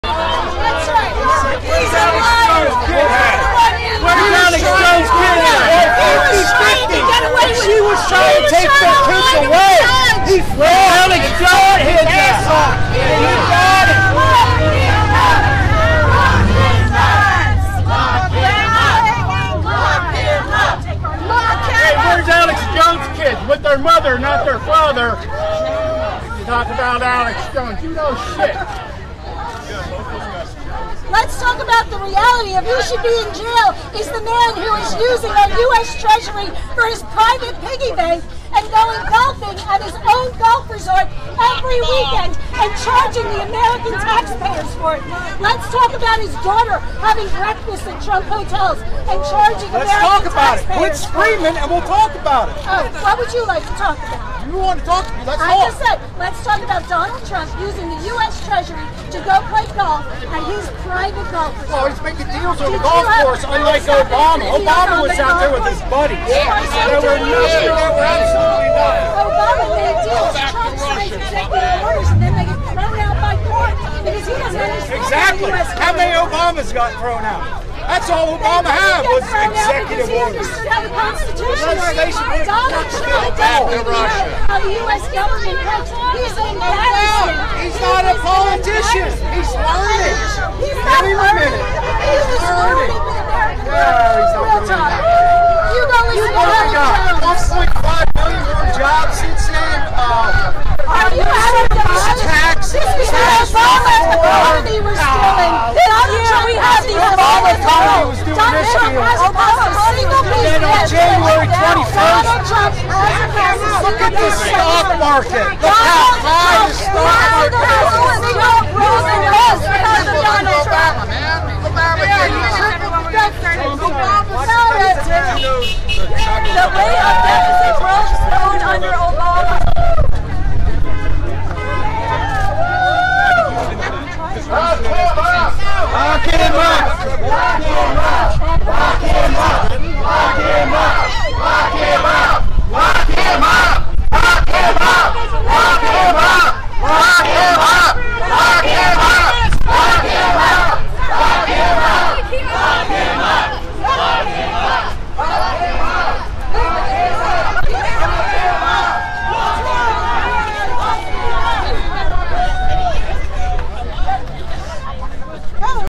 Outburst between Trump fan and protesters in Harrisburg
A Donald Trump fan confronted protesters prior to Trump's rally to mark his 100th day in office in Harrisburg on April 29, 2017.